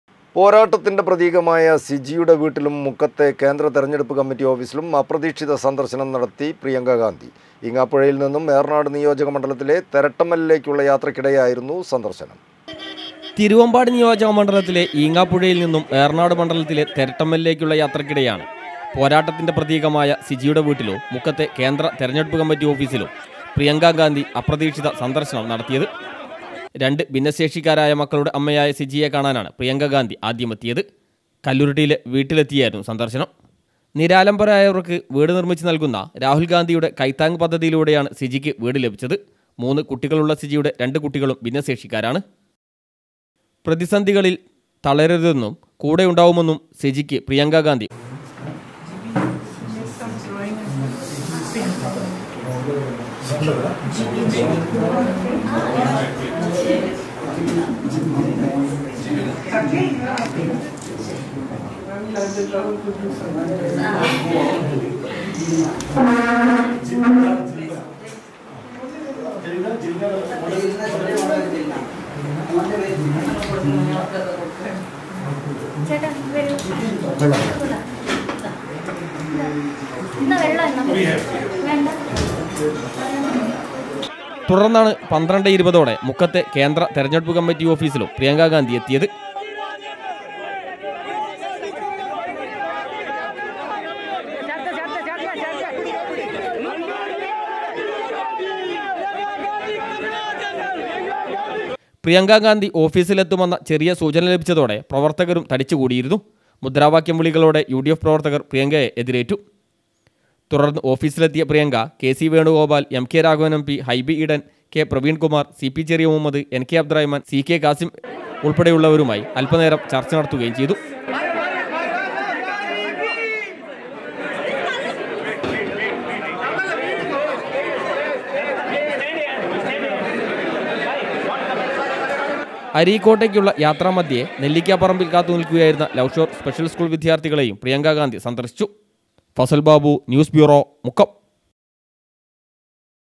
മുക്കത്തെ കേന്ദ്ര തെരഞ്ഞെടുപ്പ് കമ്മറ്റി ഓഫീസിലും അപ്രതീക്ഷിത സന്ദർശനം നടത്തി പ്രിയങ്ക ഗാന്ധി.
പോരാട്ടത്തിന്റെ പ്രതീകമായ സിജിയുടെ വീട്ടിലും മുക്കത്തെ കേന്ദ്ര തെരഞ്ഞെടുപ്പ് കമ്മറ്റി ഓഫീസിലും അപ്രതീക്ഷിത സന്ദർശനം നടത്തി പ്രിയങ്ക ഗാന്ധി.